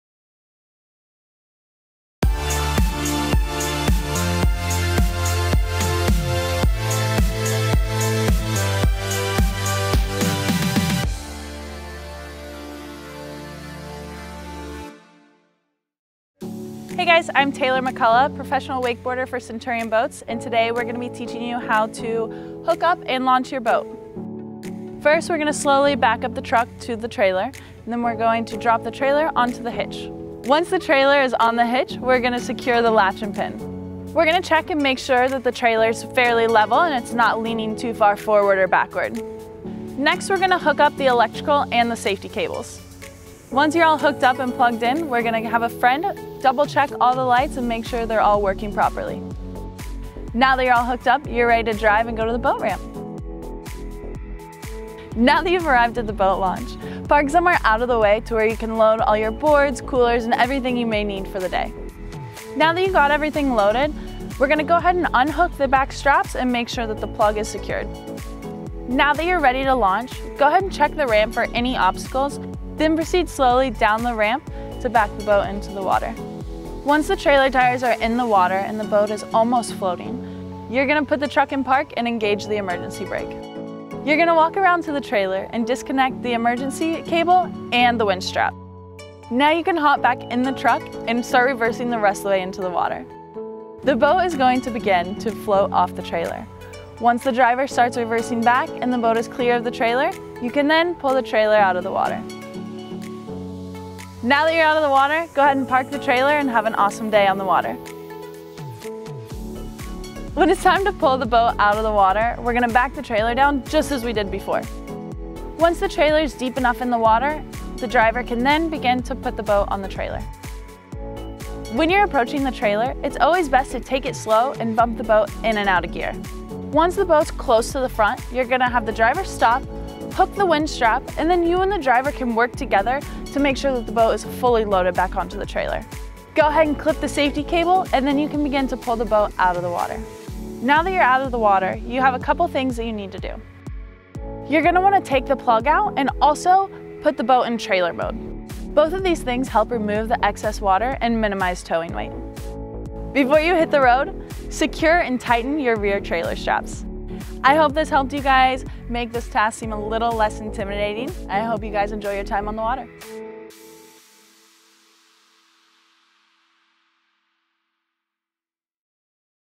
How to Trailer and Launch with Centurion Boats
Here's how to hook up and launch your boat properly with professional wakeboarder for Centurion Boats, Taylor McCullough.